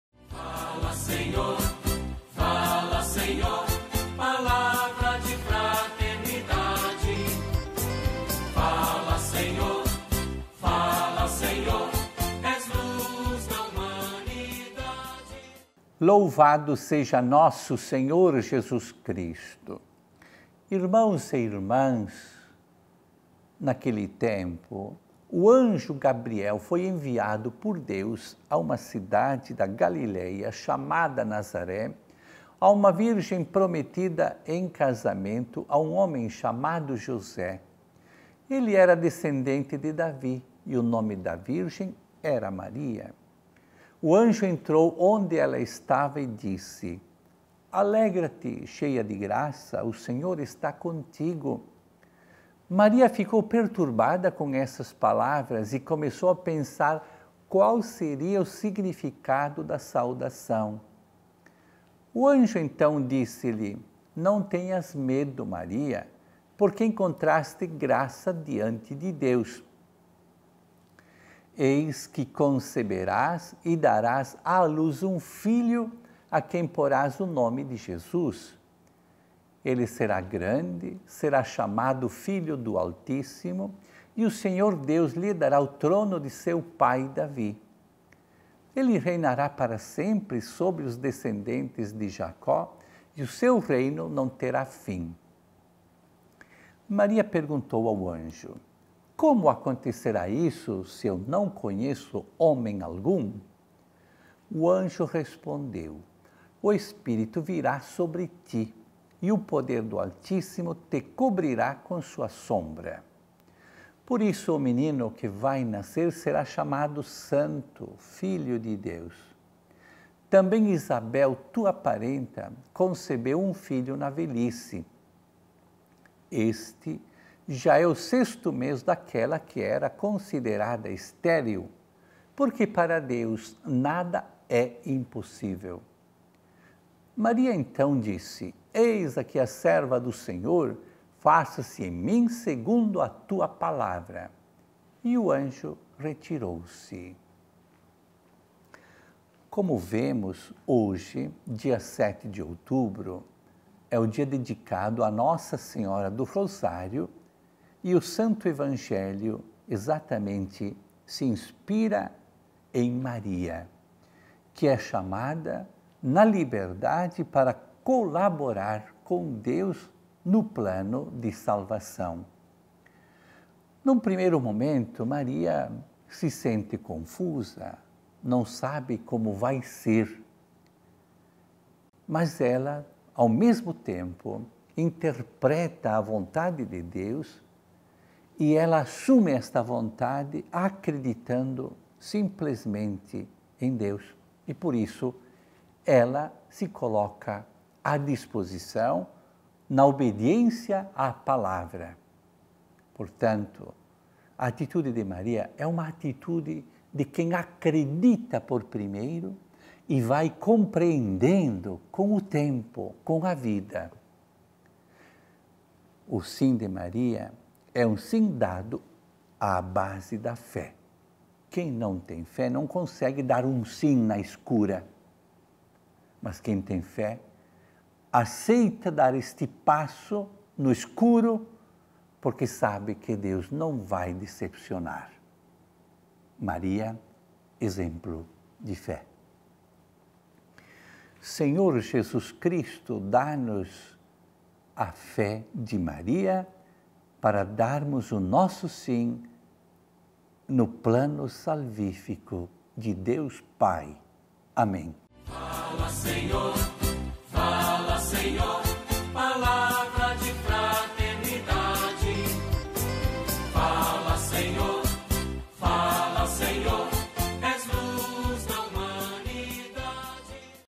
Evangelho do dia (07.10) com Dom Neri José Tondello
Reflexão do Evangelho de hoje (Lc 1,26-38) com Bispo da Diocese de Juína-MT, Dom Neri José Tondello.

Gostou do vídeo? Não esqueça de curtir e deixar seu comentário abaixo!